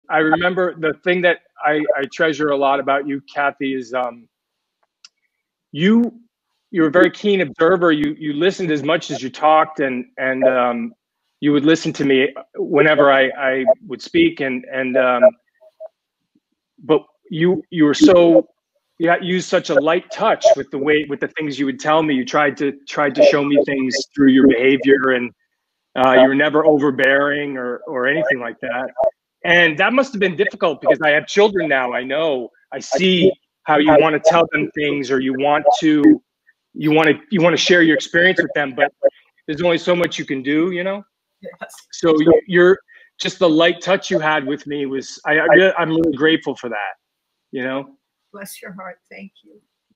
Scott DeFreitas on working with Kathryn Hays
As the World Turns interview with Don Hastings, Kathryn Hays and Scott DeFreitas (May 15, 2020).